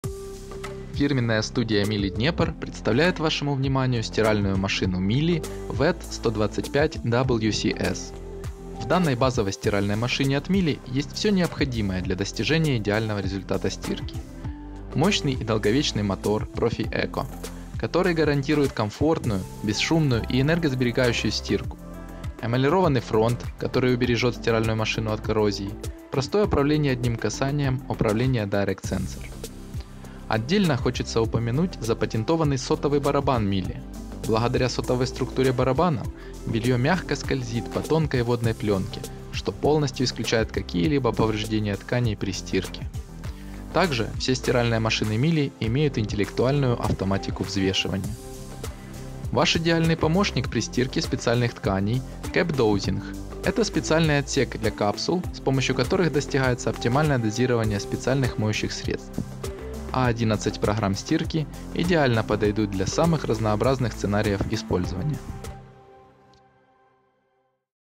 Обзор на стиральную машину Miele WED 125 WCS | Фирменная студия Miele в Днепре
Стиральная машина с фронтальной загрузкой Miele.

    Сотовый барабан Miele
    Мотор ProfiEco
    CapDosing
    Отжим 1400 об/мин
    Загрузка 7 кг
    Дисплей EasyControl
    Отсрочка старта до 24 ч
    11 программ стирки

    Защита от протечек Watercontrol

📍 бул. Екатеринославский 1 (ТЦ Cascade Plaza)
📍пр. Дмитрия Яворницкого 76
📍пр. Дмитрия Яворницкого 22 (ТДЦ Атриум)